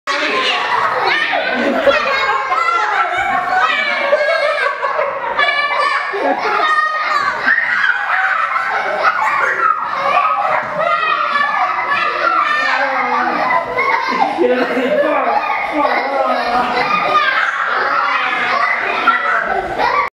Kenpo divertido Jugando con los niños
Jugando con los pequeños dragones para desarrollar su confianza y seguridad.